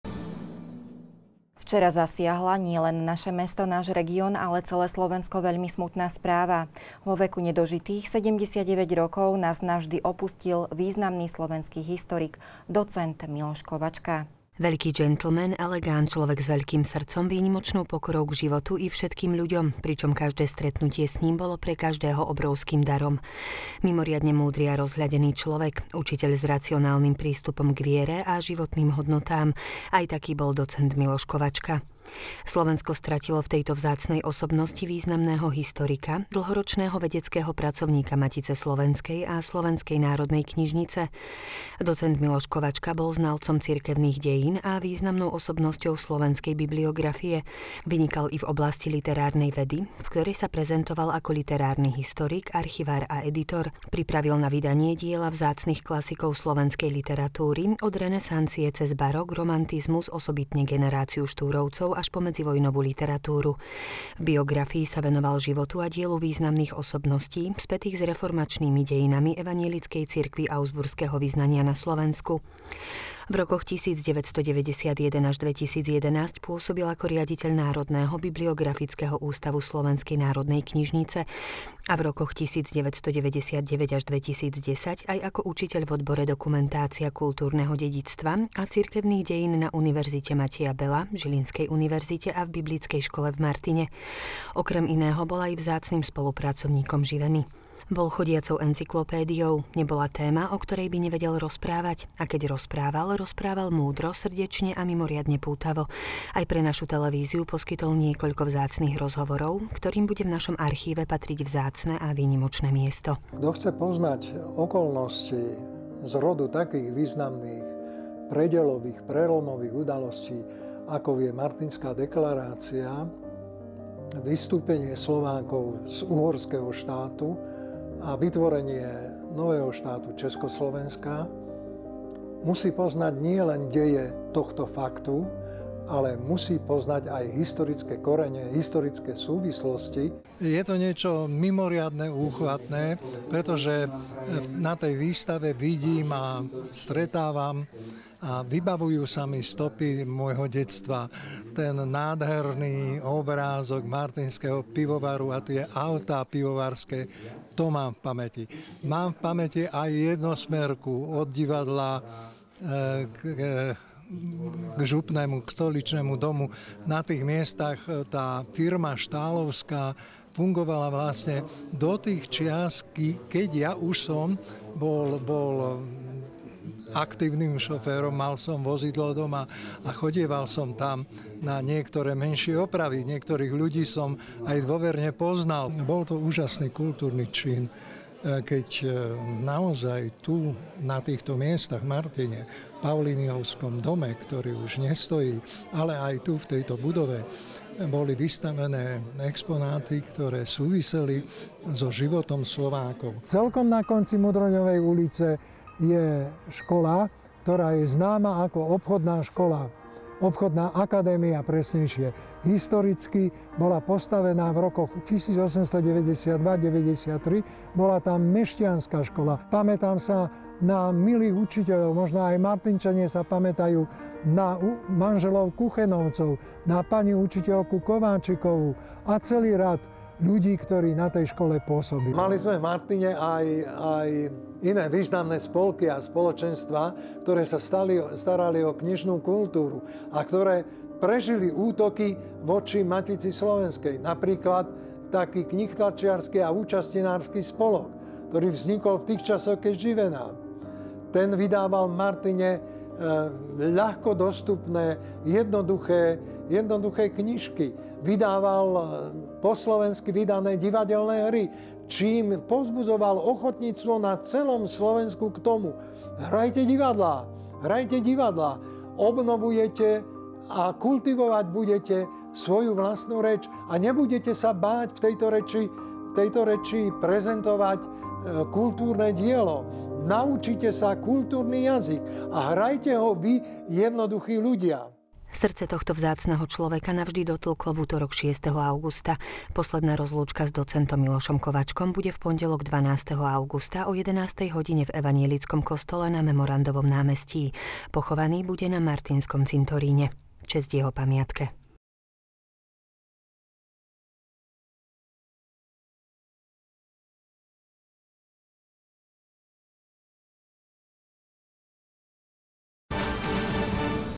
Spomienka – Miloš Kovačka
V utorok ráno (6.8.2019) prekvapila viacerých Martinčanov správa, že vedecký pracovník Matice slovenskej a Slovenskej národnej knižnice Miloš Kovačka odišiel na večnosť. Ostalo po ňom prázdne miesto aj medzi členmi evanjelickej cirkvi augsburského vyznania a tiež na Žilinskej univerzite, kde dlhodobo prednášal slovenskú literatúru, kultúrne dejiny, bibliografiu a dokumentológiu.